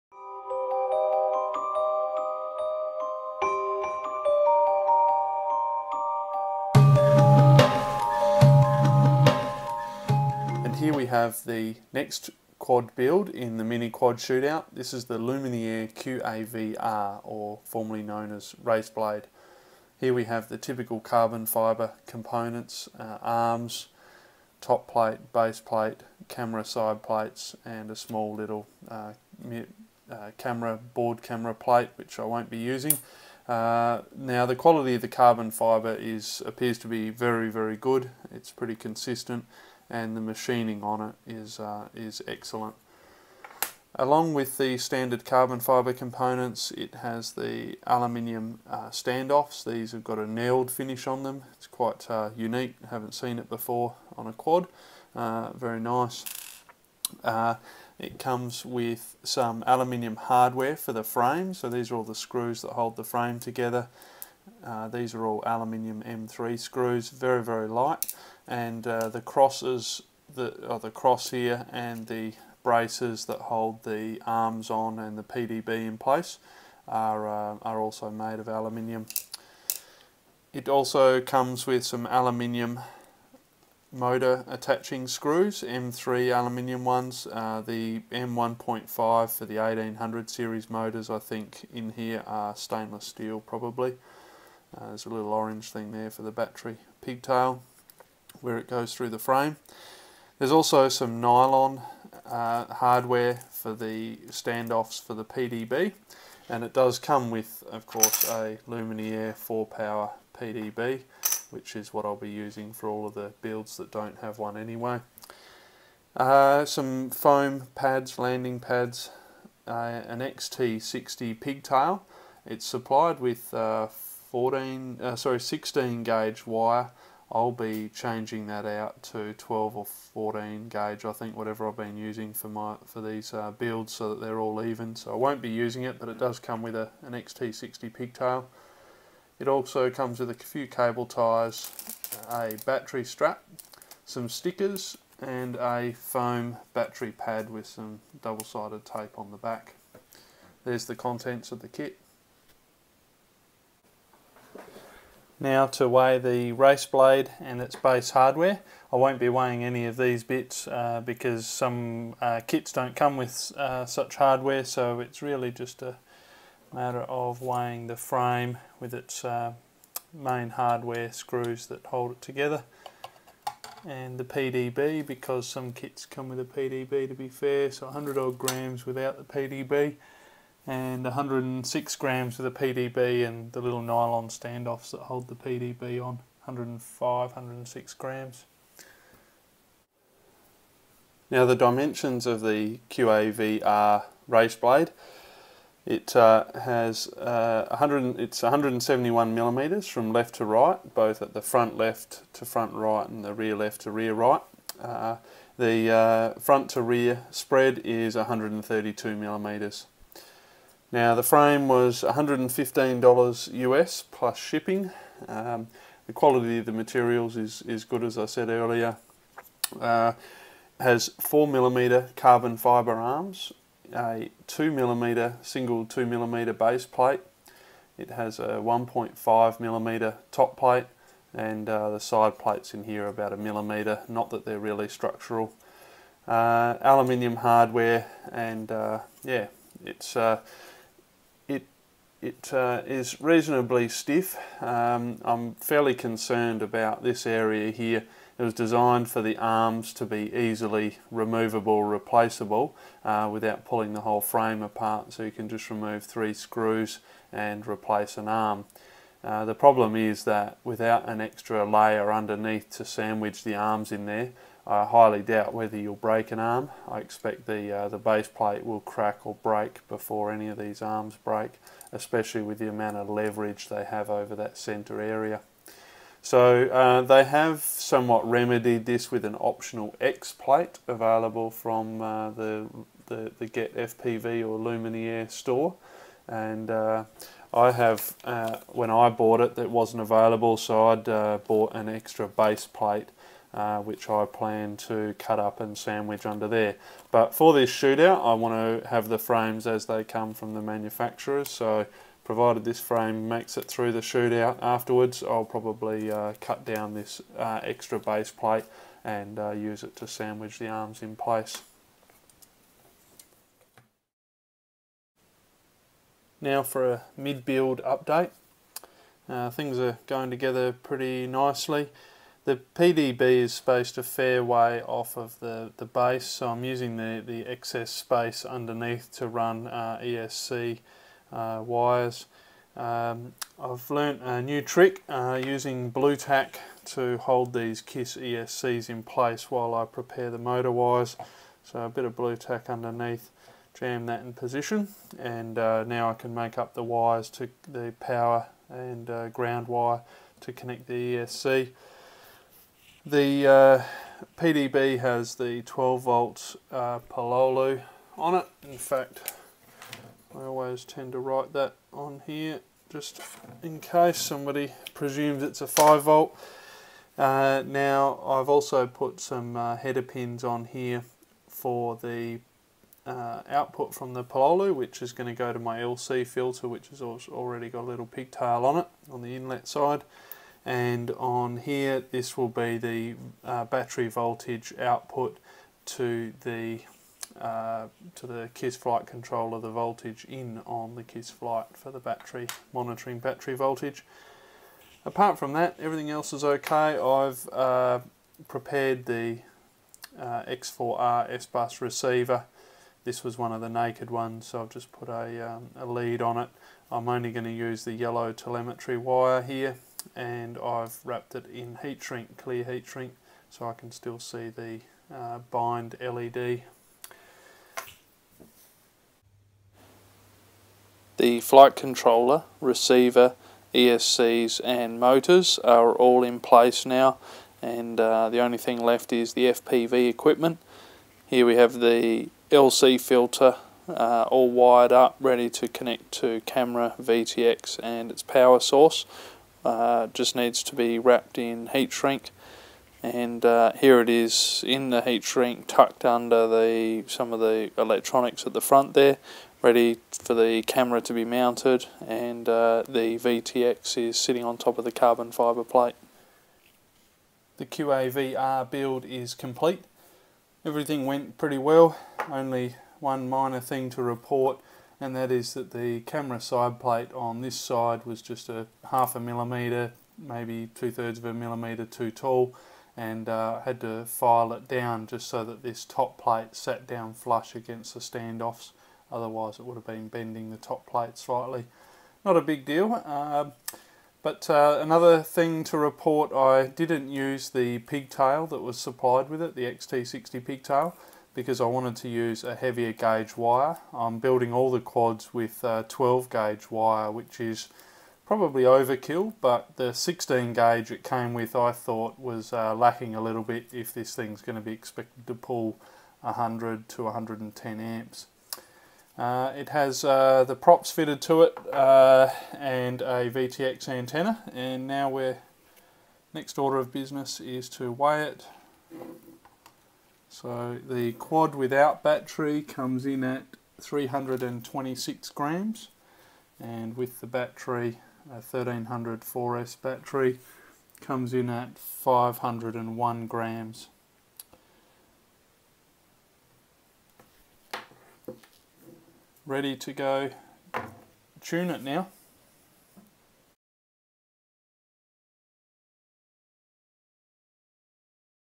Part 5 - 5"MQSO - Lumenier QAV-R Build Summary
This is the second build summary video in the 5" Mini Quad Shoot Out series.

Update: as John Nordstrand pointed out, the QAV-R now ships with a 2mm top plate and steel screws. So the frame is stronger but also heavier - they still claim 106g with PDB on the website but that was before these changes.

Lumenier QAV-R:
Price: US$115 + shipping
Front to rear motor spacing: 132mm
Left to right motor spacing: 171mm
Frame weight (with PDB): 106 grams
All Up Weight (without battery): 326 grams
All Up Weight (with 4S 1300 Graphene battery): 501 grams